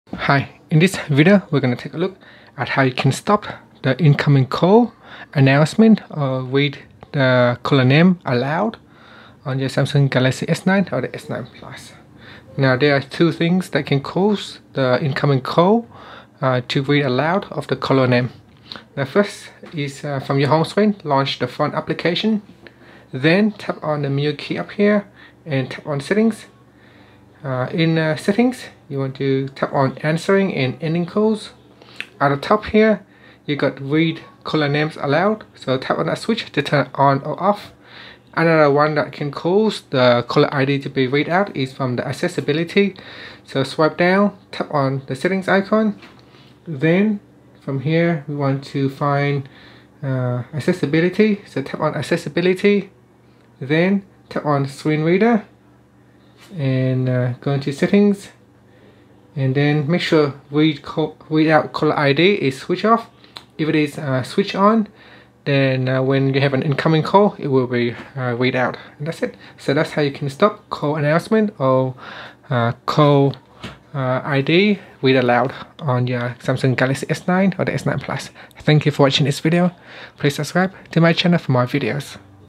Galaxy S9 / S9+: How to Stop Incoming Call Announcment Read Aloud Caller Name / ID
Learn how you can stop incoming call announcement read aloud caller name or ID on Galaxy S9 / S9+.